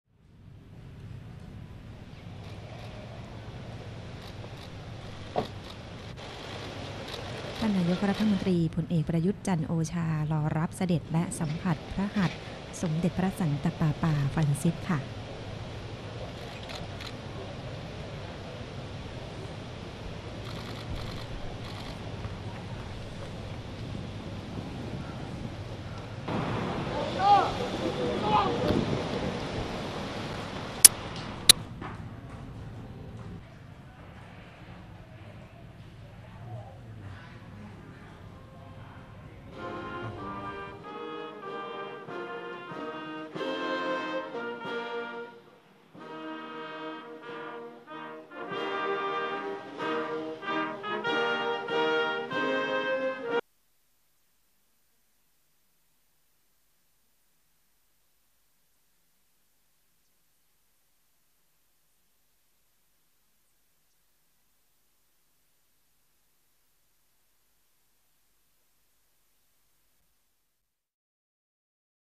Le pape François rencontre le Premier ministre thaïlandais | AFP Images
Le pape François a rencontré le Premier ministre thaïlandais Prayut Chan-O-Cha à Bangkok, à l'occasion d'une visite du souverain pontife dans le pays.